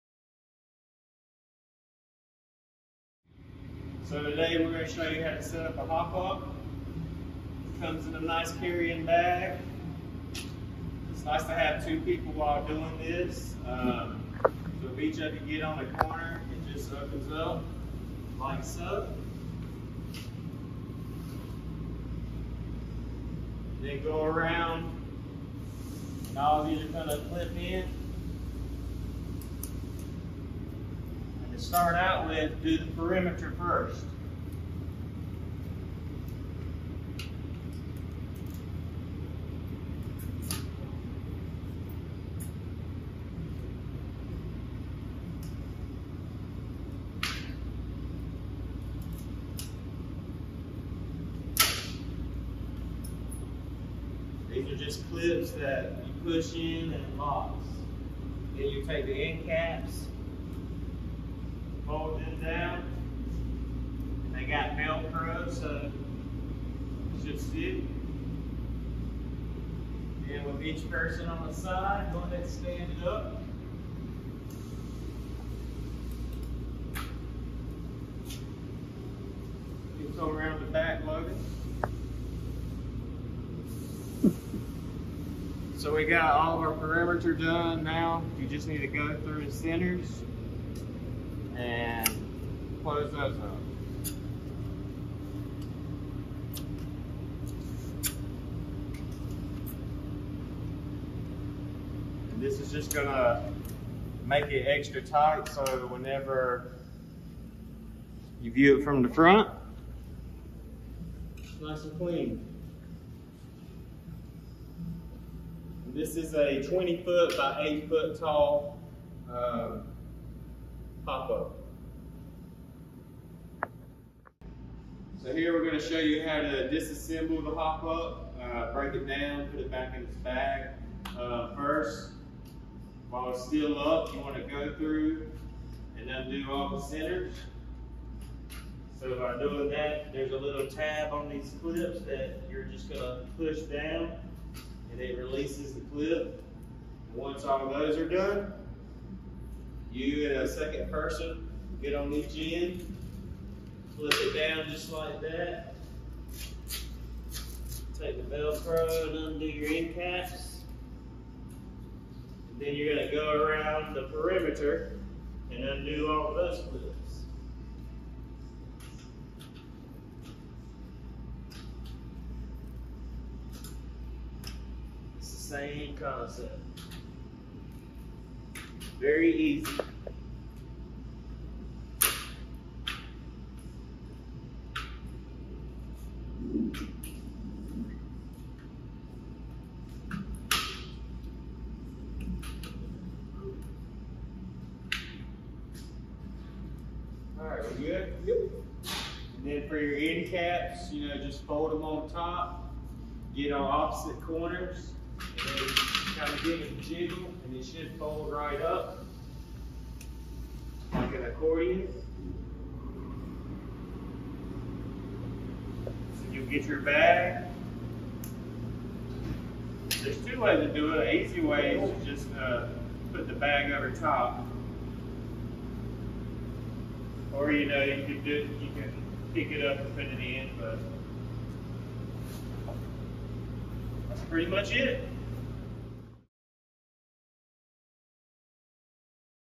RPL Backdrop Assembly | Heritage Signs & Displays
In this step-by-step tutorial, we guide you through the assembly process of the RPL Backdrop from Heritage Signs & Displays. Whether you're setting up for a trade show, conference, or any event, our easy-to-follow instructions will help you assemble your backdrop quickly and efficiently. Perfect for creating a professional display, the RPL Backdrop is designed for durability and ease of use. Watch now to learn how to set up your backdrop and make your next event stand out!

Our Locations